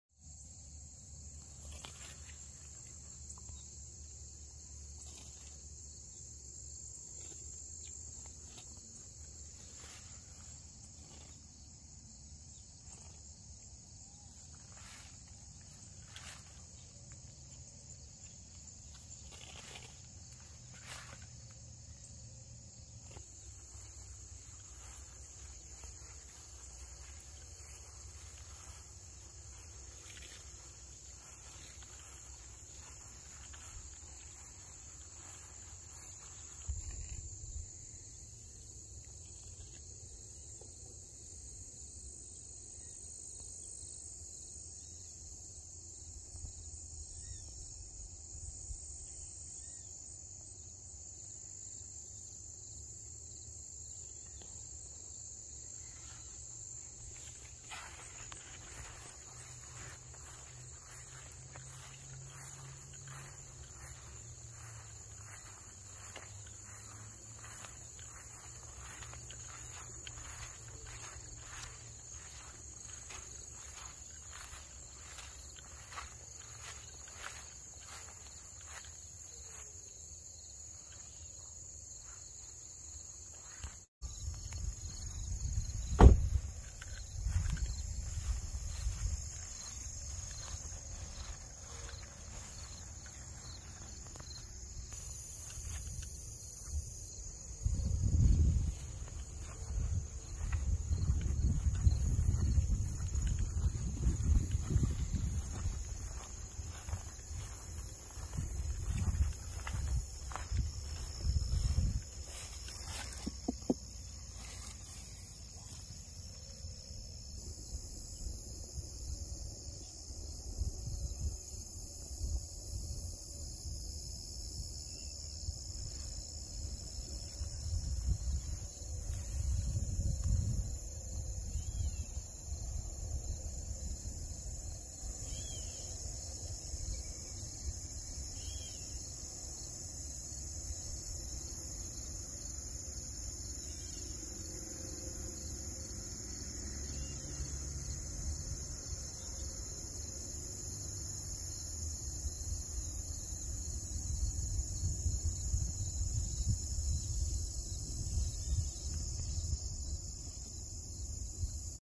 garden update September 1 2021
pumpkin progress first of September in Ohio after hurricane Ida massive rainfall